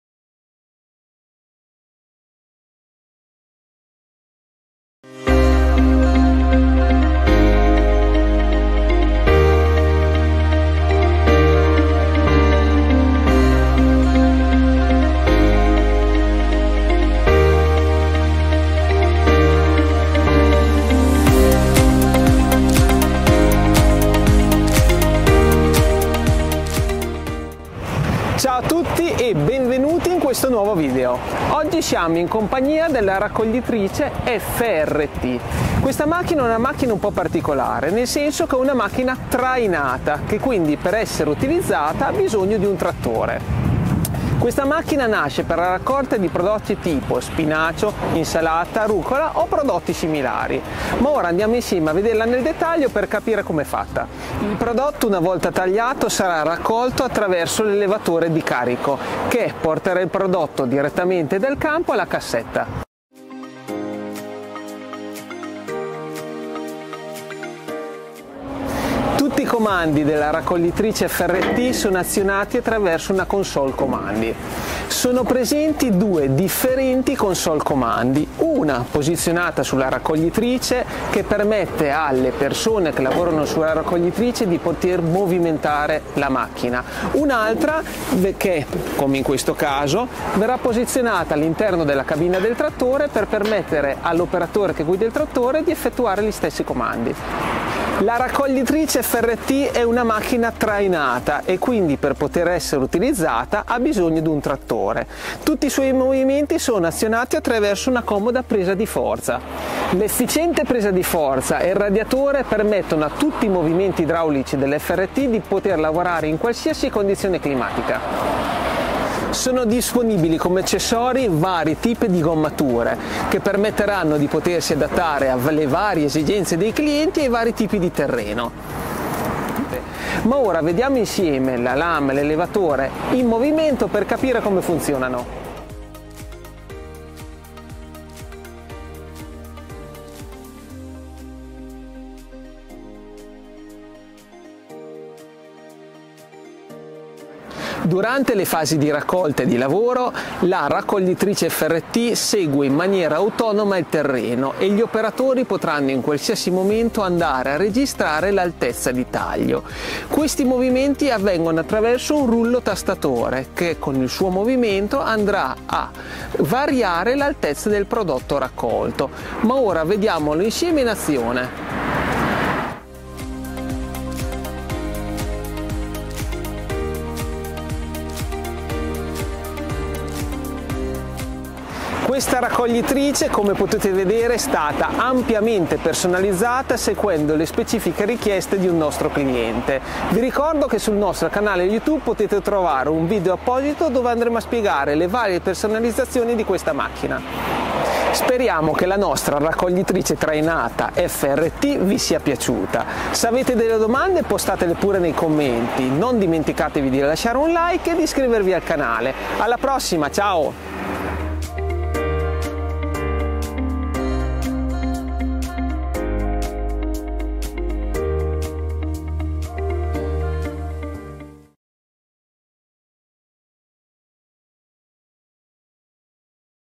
DePietri FRT Harvester
The FRT trailed harvester is especially suitable for picking up fresh vegetables such as: Salad, Parsley, Spinach, Rocket.